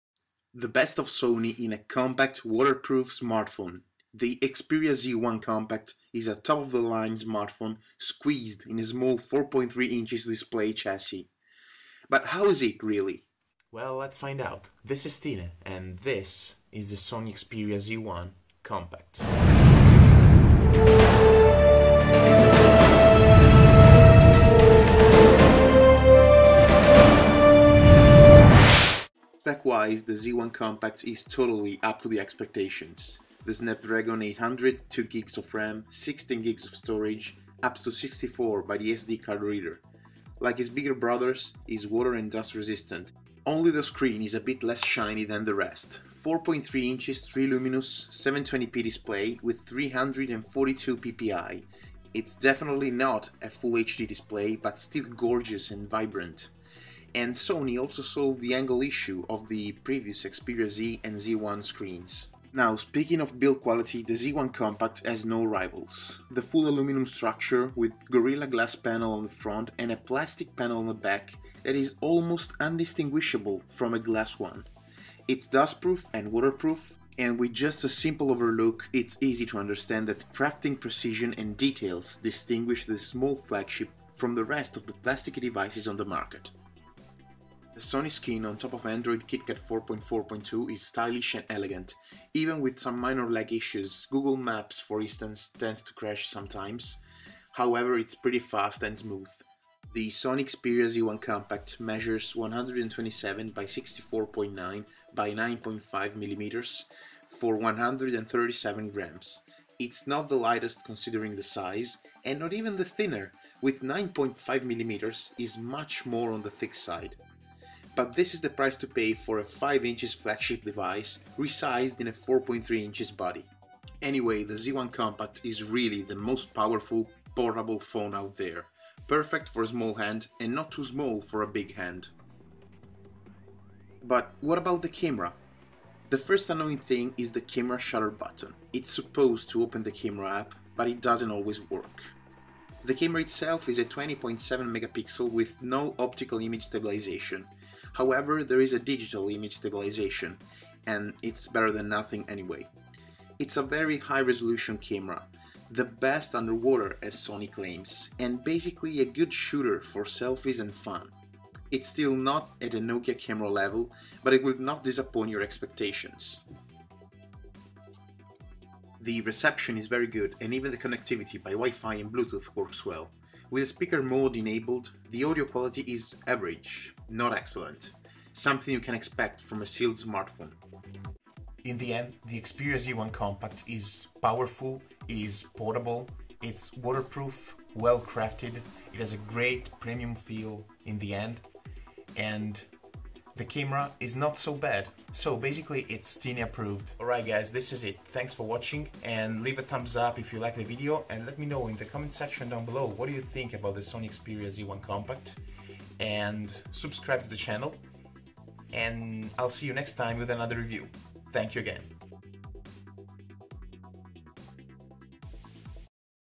Sony Xperia Z1 Compact - Full Review
Today we take a look to the Sony Xperia Z1 Compact, the Smallest Flagship device until now. Let's find out if it's worth  to get it.